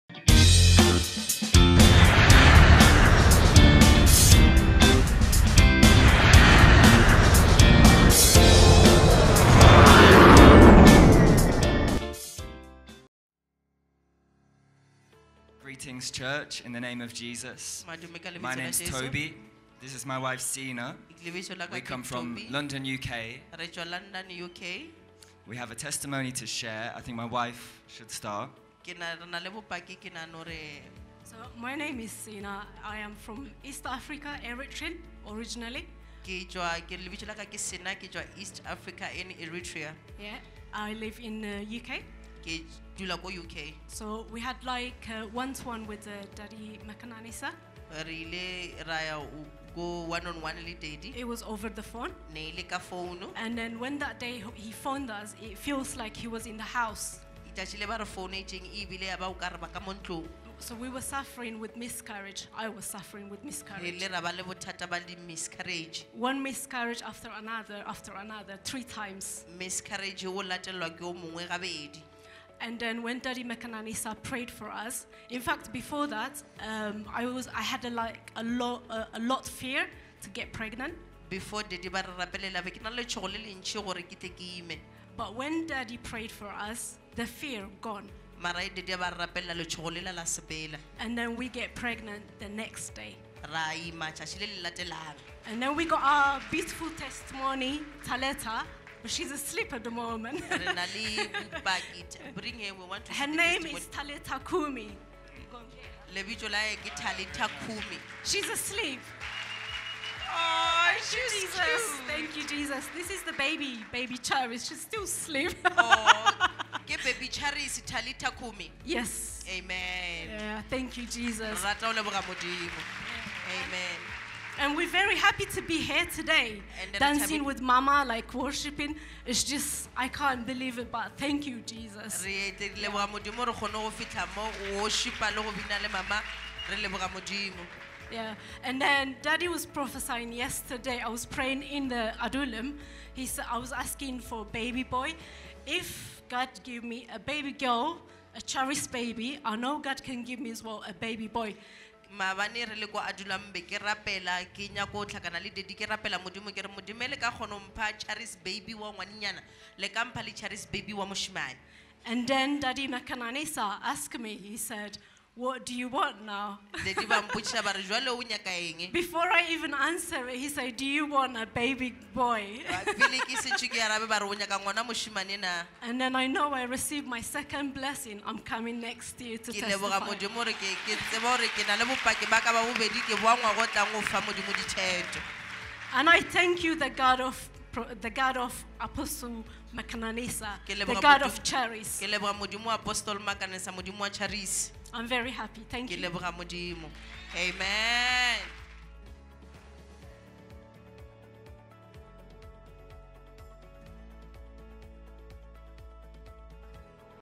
Multiple Miscarriages end AFTER ZOOM ONE ON ONE with Apostle BJ Makananisa
Listen to this Wonderful Testimony from a couple living in the United Kingdom, as they explain how God blessed them with a Baby Girl after their Zoom one on one session with Apostle BJ Makananisa